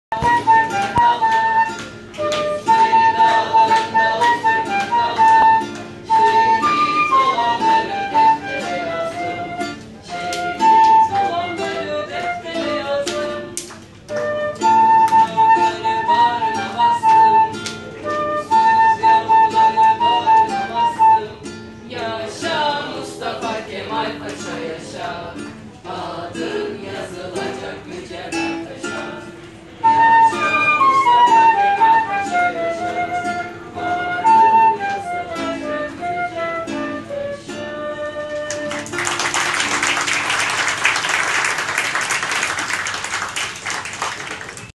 GESTAŞ VAPURUNDA CUMHURİYET COŞKUSUU
Türkiye’nin ve Çanakkale’nin her yerinde coşkuyla kutlanan 29 Ekim Cumhuriyet Bayramı, GESTAŞ vapur seferlerinde de kutlandı.